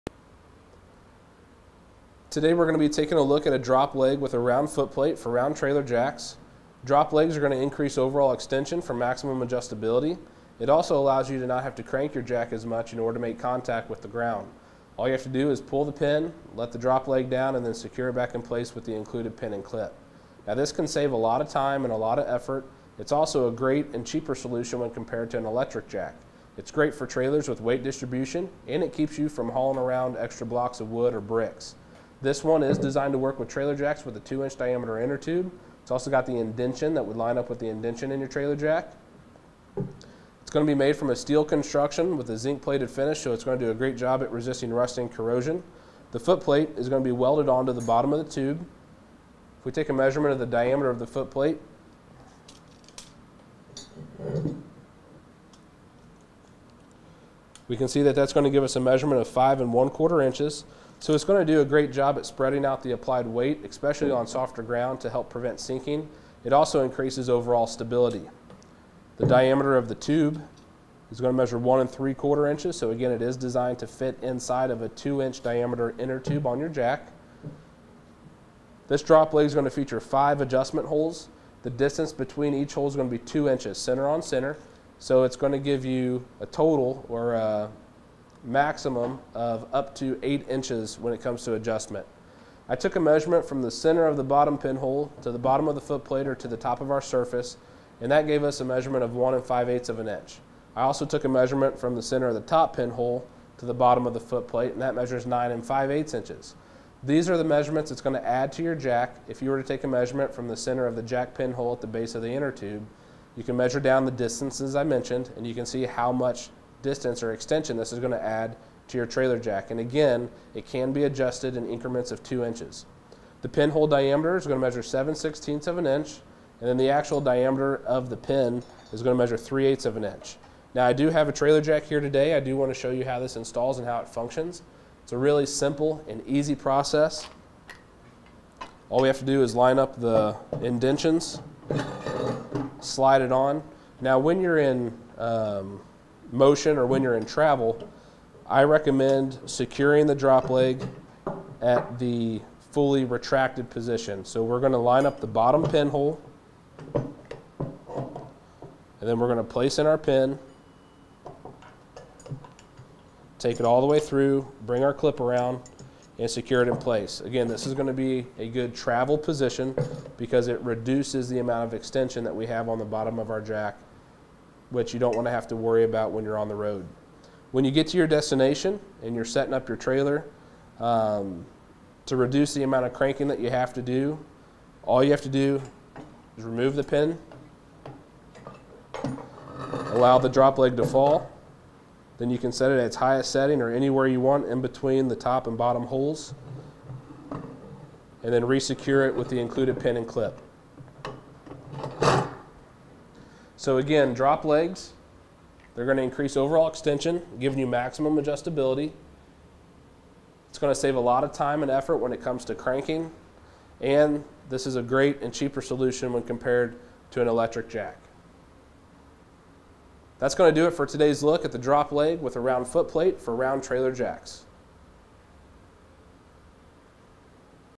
etrailer Trailer Jack JF-1005H-Z Review
Today we're going to be taken a look at a drop leg for round trailer jacks. Drop legs are going to increase overall extension from axle and adjustability. This particular drop leg is designed to work with around trailer jacket has a 2-inch diameter inner tube. It's made from a steel construction so it's very strong and durable and it's going to have a zinc plated finish which is going to make it resistant from rust and corrosion. It does come with the pin and clip, secure the drop leg to the trailer jack, and you can set it to whatever position is going to work best for your application. The pin hole diameter on the unit's going to measure just over three eighths of an inch in the actual diameter is right at three eighths of an inch. Here at the bottom of the driveway does a welded on foot plate.

This foot plate is going to give us a diameter of five and one quarter inches, that's from outer edge to outer edge, so it's going to do a great job of spreading out the applied weight even on soft ground to help prevent sinking. It's also going to help increase overall stability. Now a few other measurements that I want to mention for you real quick. We're going to take a look at the tube diameter. Measuring this piece right here, outer edge to outer edge we're looking at one and three quarters of an inch. This is designed to fit inside of a 2-inch diameter inner tube on your jack.

The overall height measuring from the bottom of the foot plate to the top of the tube. That's going to give us a measurement of right around 13 and 1 quarter inches. Now this is going to offer eight inches of additional extension. We can tell that by taking a measurement from the center of this hole, to the center of this hole. If we do that we can see that center on center, those are we separated by 8 inches and is able to adjust in 2 inch increments because that's the distance between each hole center on center.

That's going to do it for today's look at the drop leg for round trailer jacks. .